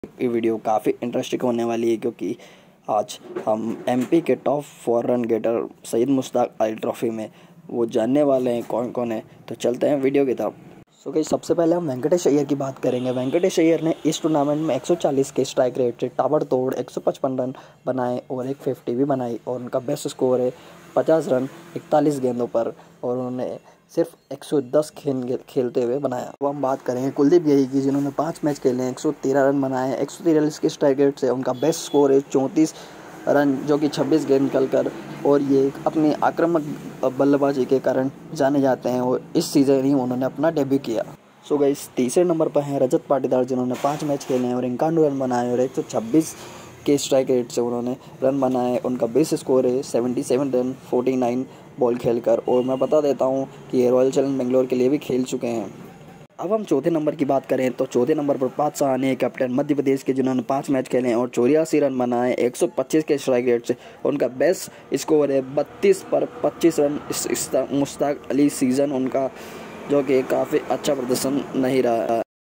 top-4 batter syed mustaq Ali trophy 2021-22
hello guys

इस वीडियो में हम बात करने वाले top batter in Syed Mushtaq Ali trophy for Madhya Pradesh

andhra pradesh
uttar pradesh